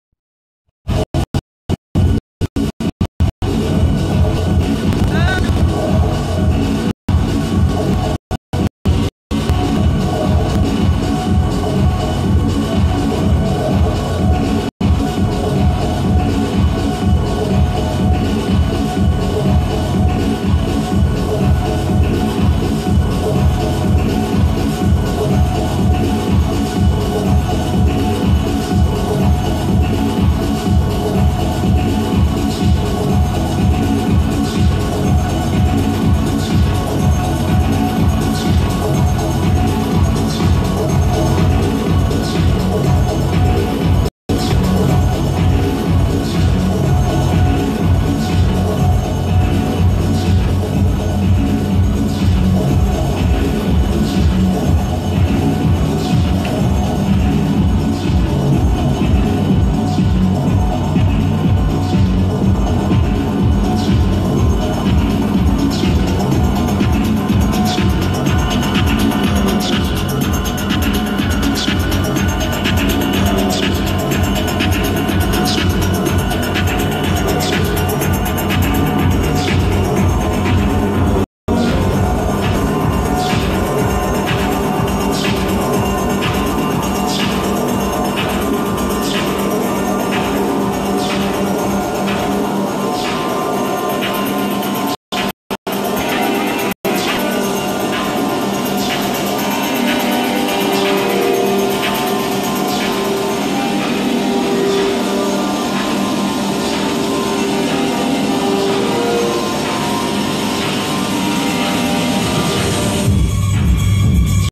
Event Design Boston
Looking for event design around Boston? SYB Event Planning can transform any space, such as in this pre-event setup sneak peek of 7th Annual Harvard Masquerade Ball's "Winter Wonderland" at Sheraton Boston Hotel. Thanks to Jennifer Little for the video!